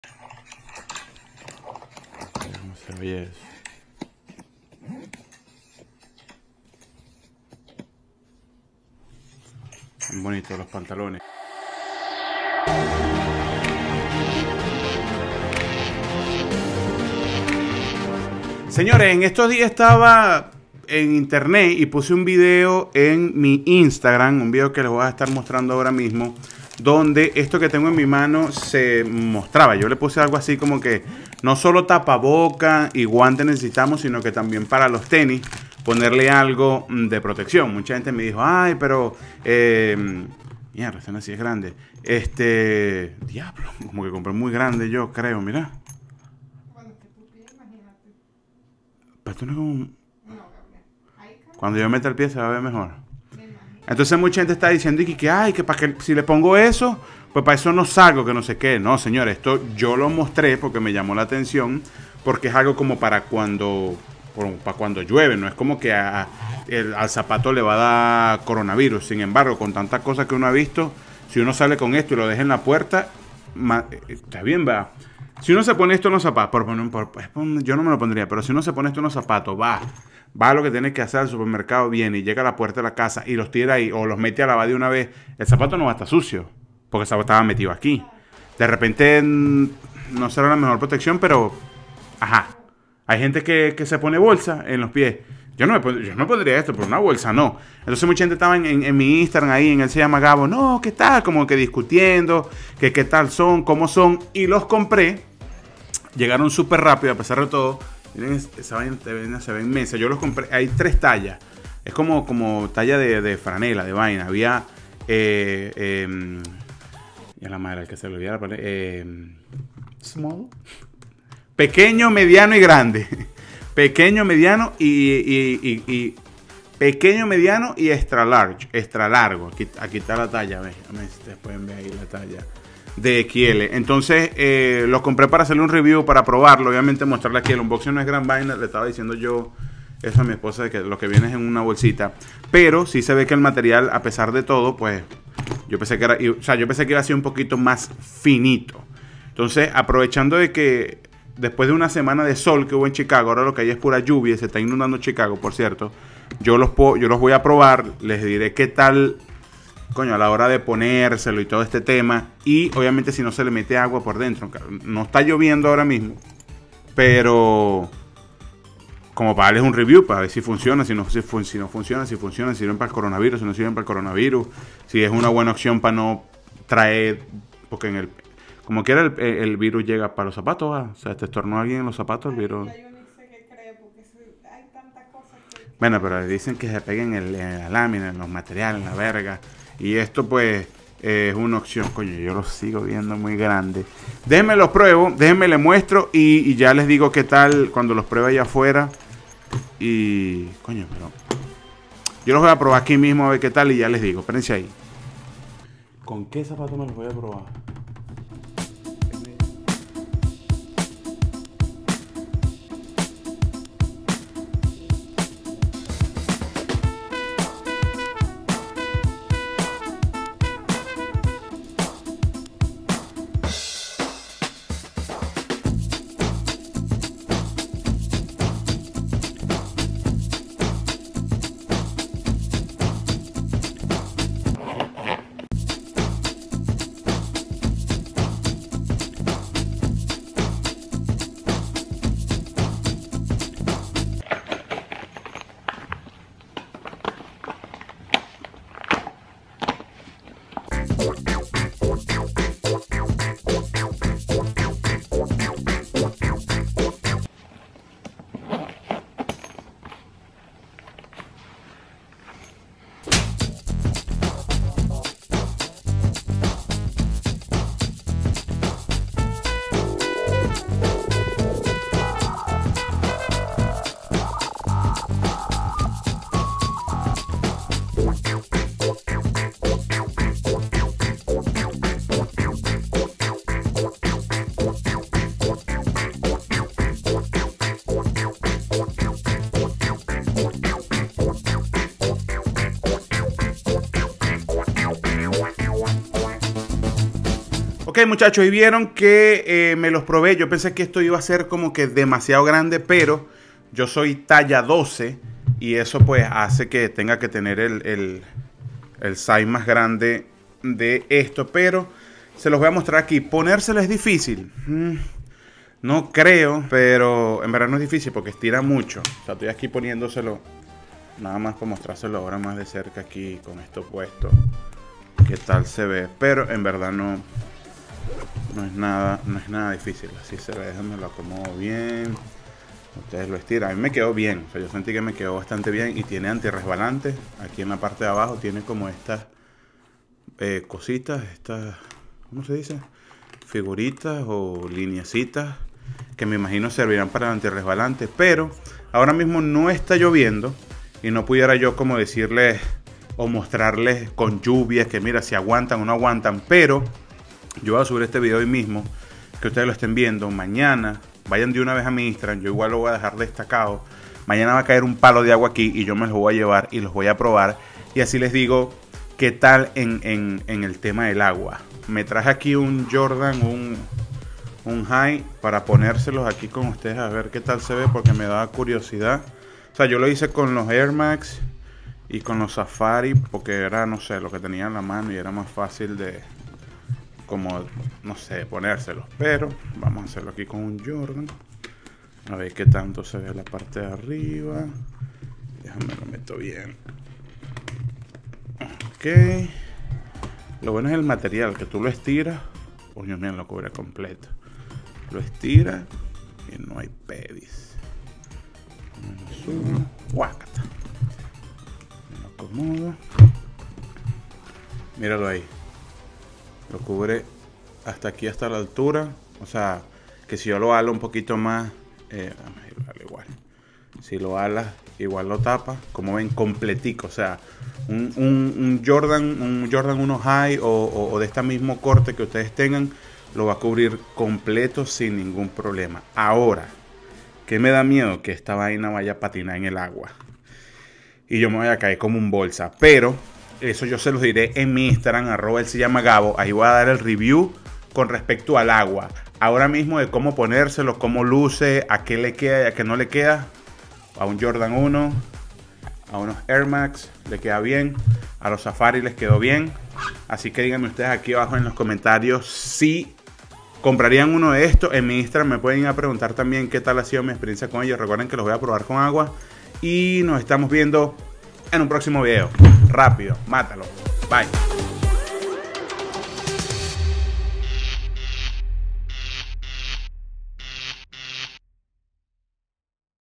ESTOS PROTECTORES PARA LA LLUVIA ESTAN INCREIBLES!!!!!!!
Muchachos este video es para mostrarle estos protectores que causaron mucha polemica en mi instagram, los voy a estar usando en estos dias lluviosos y en mi instagram les dire que tal!